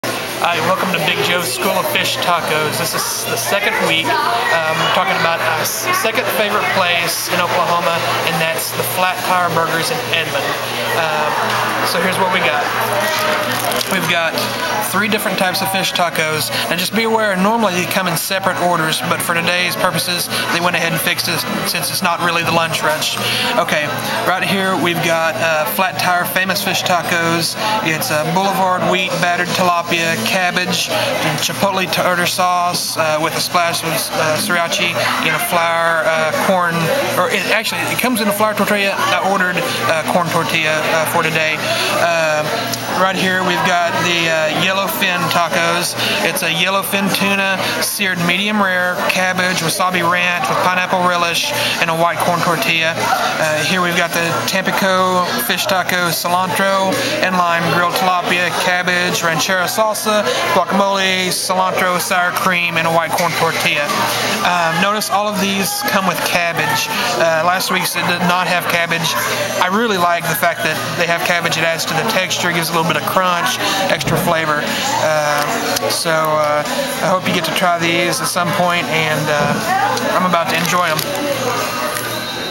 "Big Joe's School of Fish Tacos" -- week two
Joe Eatherly, of "Big Joe's School of Fish Tacos", shares his second favorite fish taco found in the state of Oklahoma. The restaurant responsible for the dish, Flatire Burgers, is located in Edmond, Oklahoma. to learn more about fish tacos through the series, "Big Joe's School of Fish Tacos".